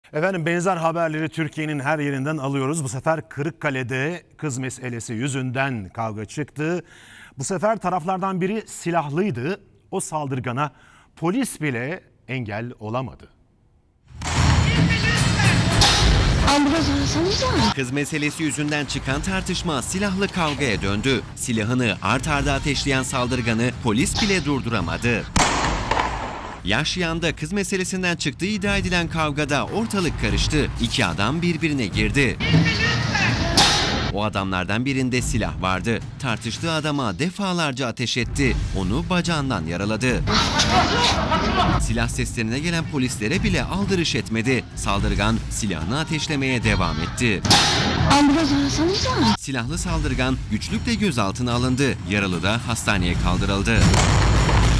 Kız meselesinde kan aktı!
Kırıkkale'nin Yahşihan ilçesinde kız meselesi yüzünden iki kişi arasında çıkan silahlı kavgada 1 kişi bacağından vurularak yaralandı.